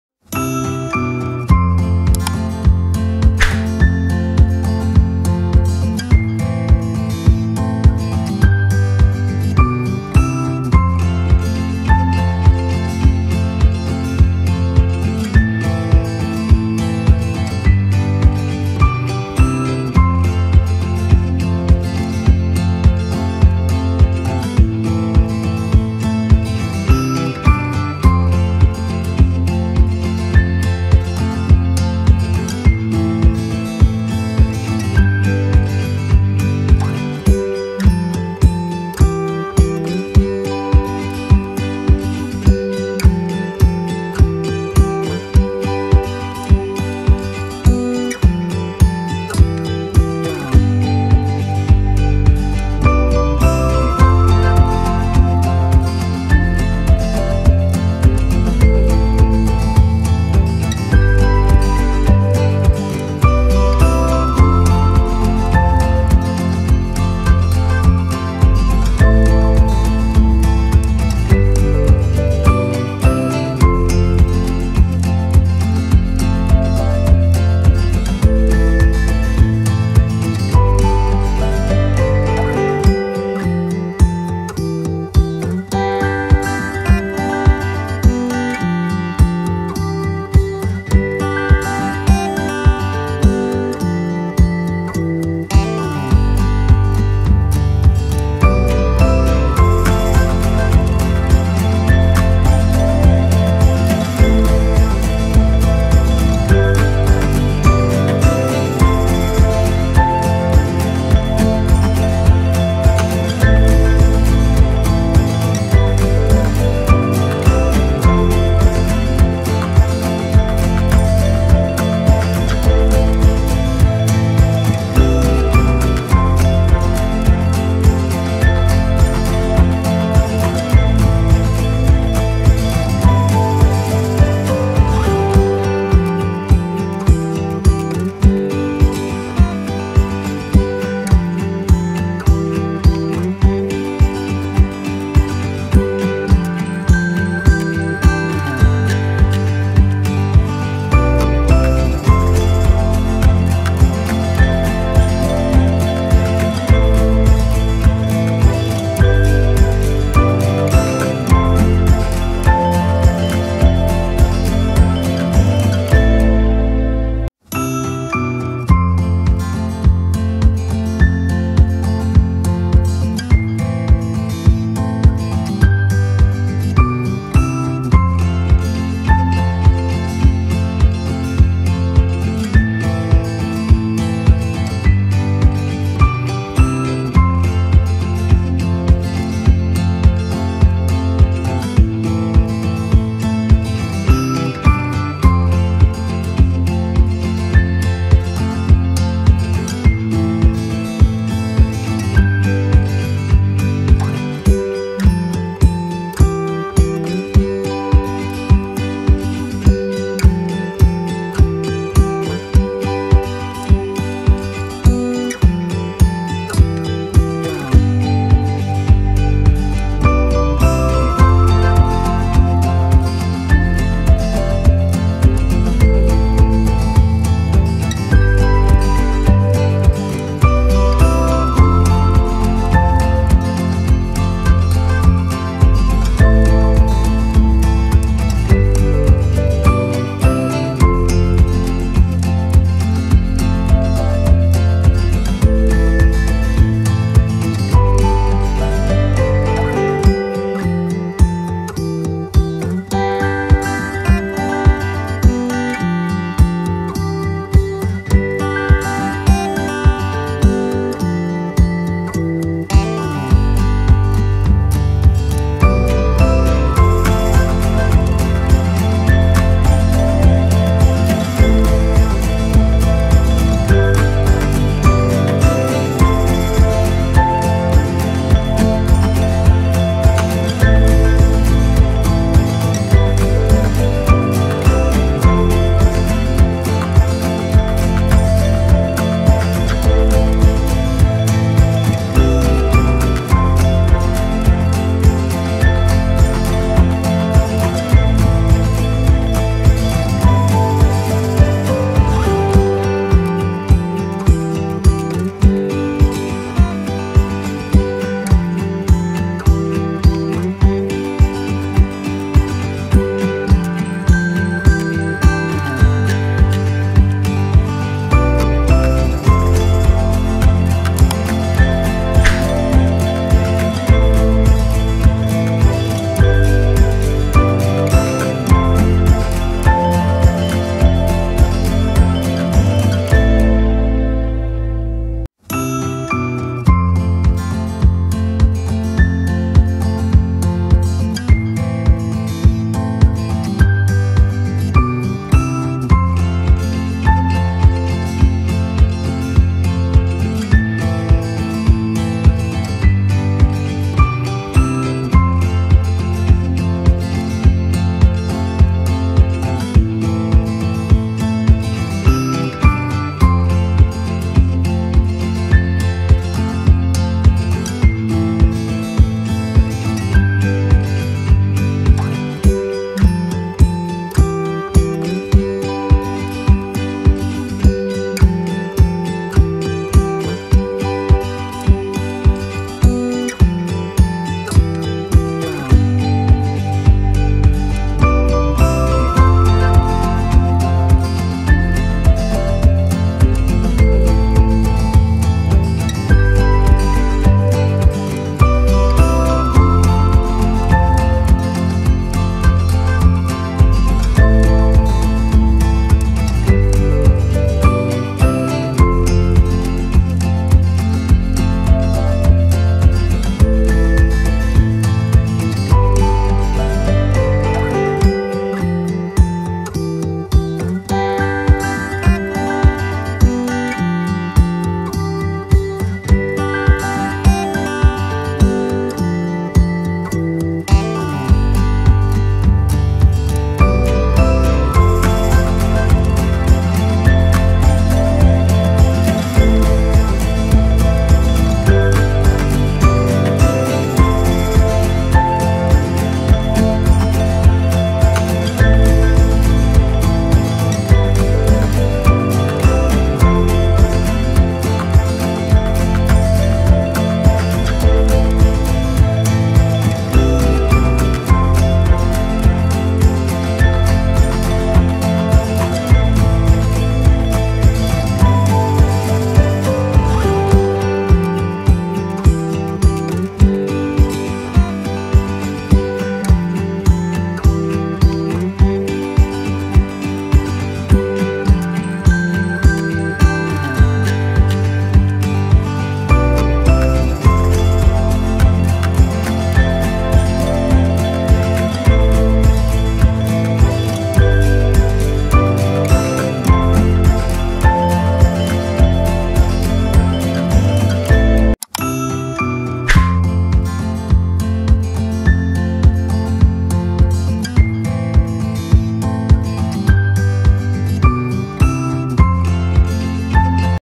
Easy Way to Sewing Neck With Frill Women's | Useful Sewing Hacks and Lovely
Hi guys, this is very easy , trust me
if you have any question or if you used this pattern like me, be sure to let me know